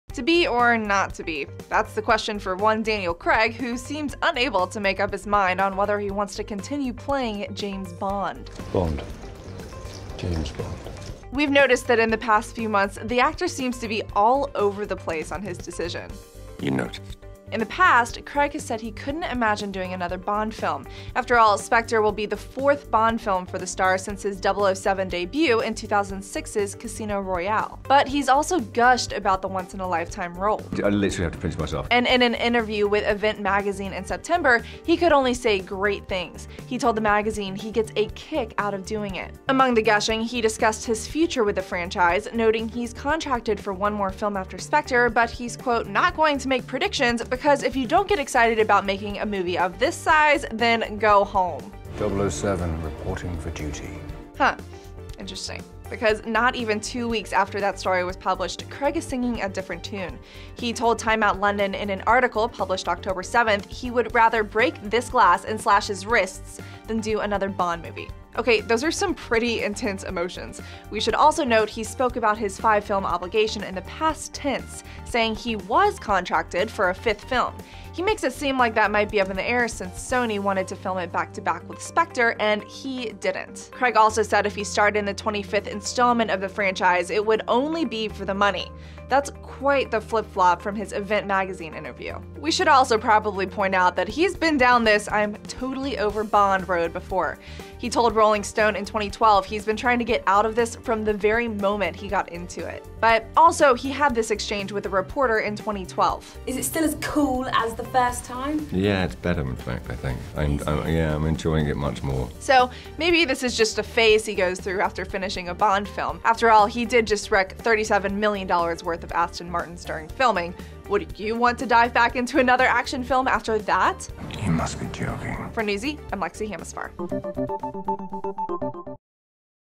Daniel Craig Is A Flip-Flopper On Shooting More Bond Films - Newsy
Daniel Craig seems to be on the fence about appearing in more James Bond films. For now, he's not keen on suiting up again.

Sources:
Getty Images